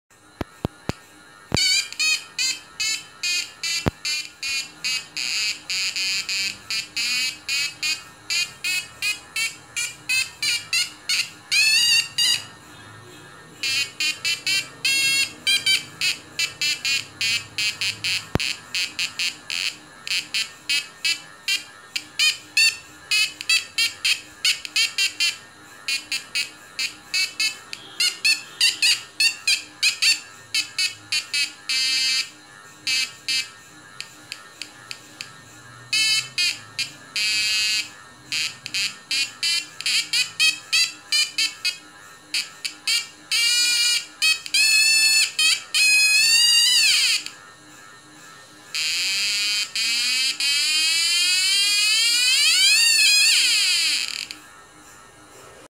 Resistance 'audio-meter' aka simple Lie Detector (Polygraph)
A fun electronics toy bssed on instructions at sciencetoymaker.org
I dont understand working of transistors in depth but I read that this is called Sziklai Pair configuration. I guess it is a fun toy for attracting kids to electronics. The CD actually has a 9V battery taped behind it and thats what I used for powering the circuit (I was trying to make a gyroscope toy from it). The wooden table has a line of my saliva! We need a conducting path between the terminals which will act like a varying resistor. I could have used water but then I was too lazy and didnt mind being gross! It wpuld be interesting to measure the resistance of that saliva and see if the speaker makes sound only for a particular resistance range or for low resistance too.